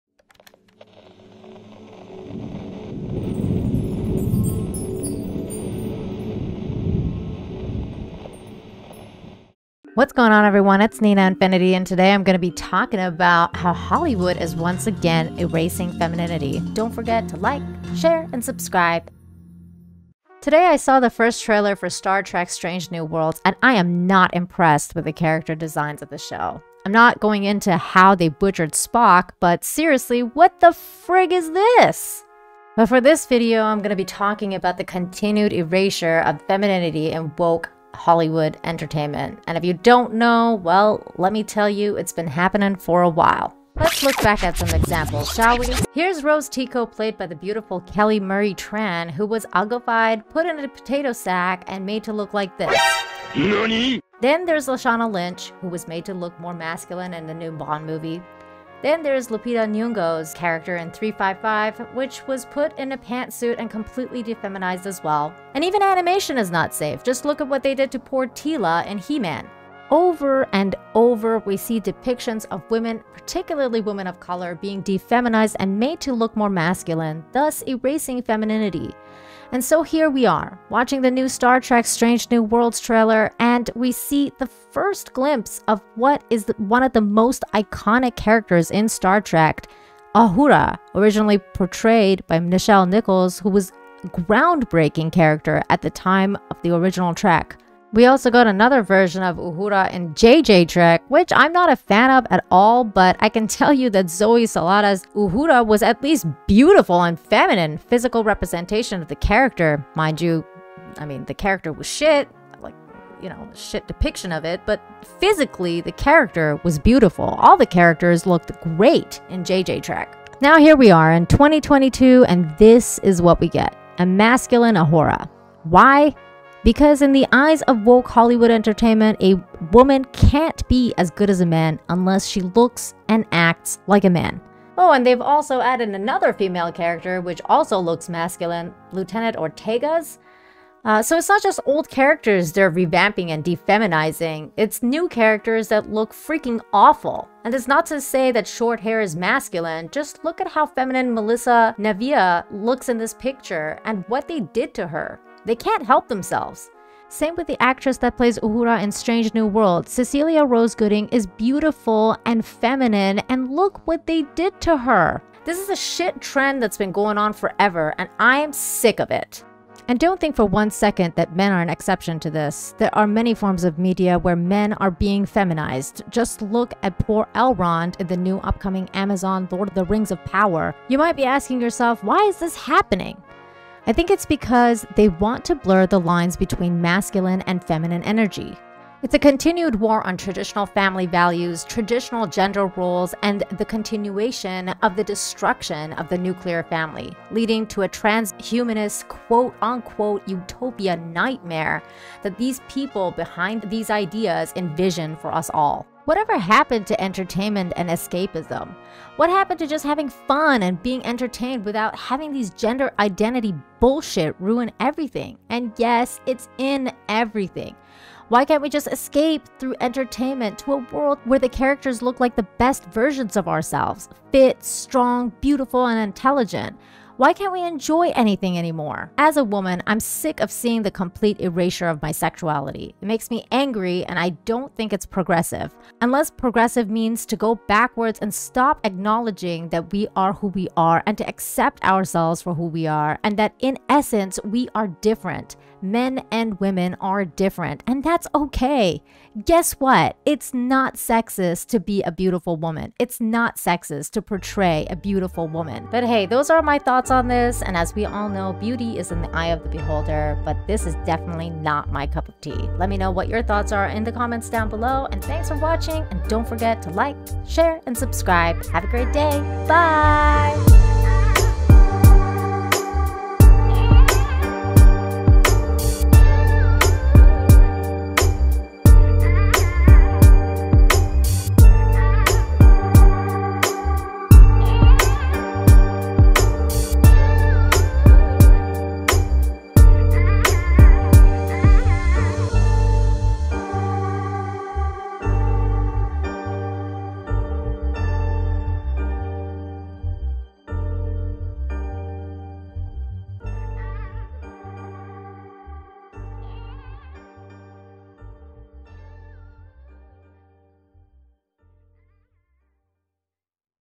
I'm Tired Of This....
My reaction to the new Star Trek: Strange New Worlds Uhura reveal. How identity politics are running everything.

Star Trek: Strange New Worlds will be airing on Paramount+/CBS All Access.